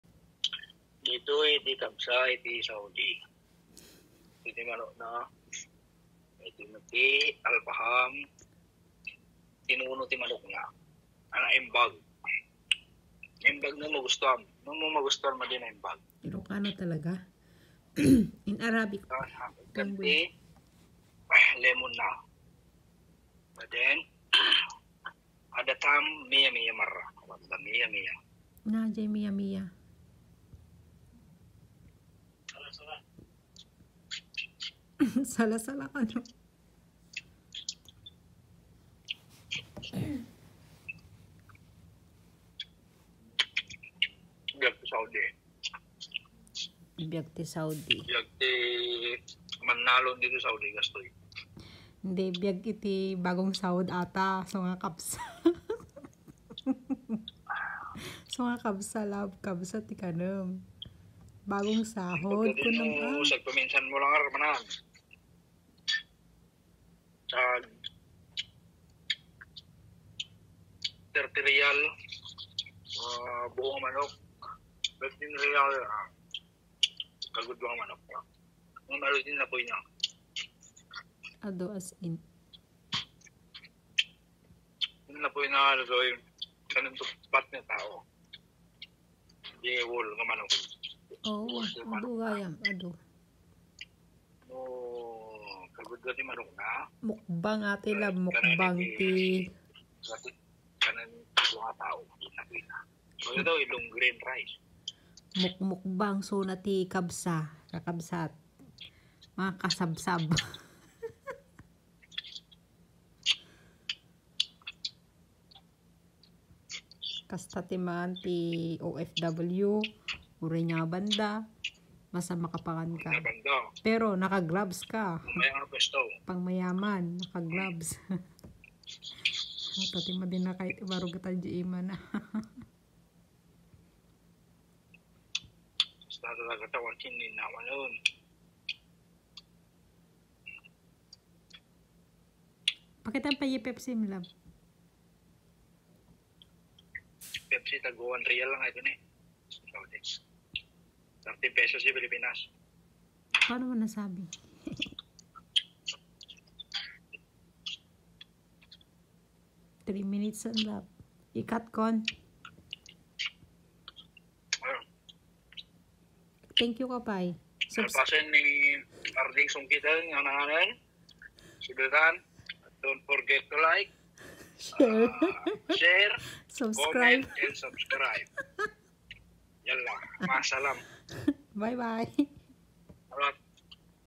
KABSA MUKBANG SAUDI ARABIA (Ilokano Language)
Kabsa is a mixed rice dish, served on a communal platter, that originates from Saudi Arabia but is commonly regarded as a national dish in countries of the Arabian peninsula. The dish is made with rice and meat.